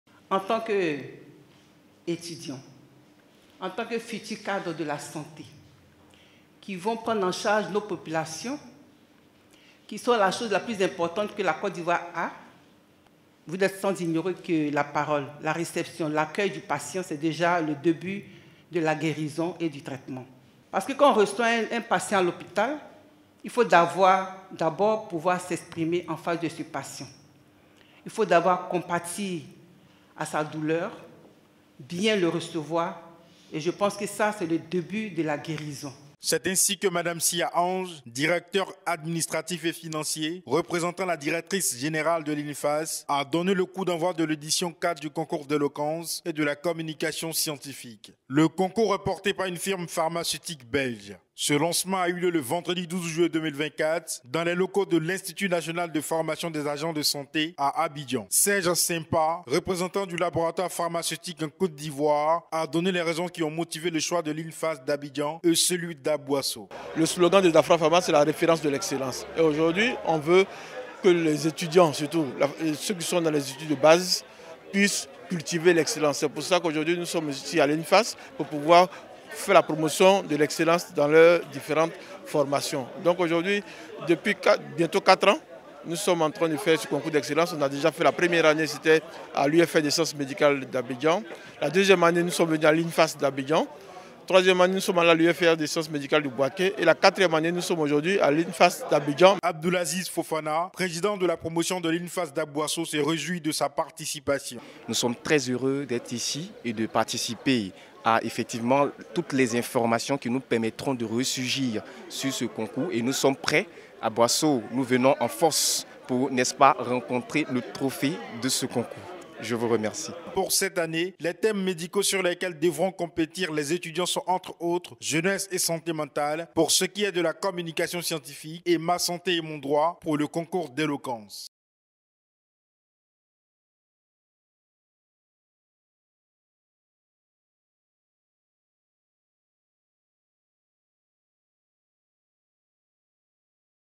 Excellence à l’Infas : La quatrième édition d’un concours lancée
C'est ainsi que Mme Sia Ange directeur administratif et financier représentant la directrice générale de l'Infas a donné le coup d'envoi de l'édition 4 du concours d'éloquence et de la communication scientifique. Le concours est porté par une firme pharmaceutique belge. Ce lancement a eu lieu le vendredi 12 juillet 2024 dans les locaux de l'institut national de formation des agents de santé à Abidjan.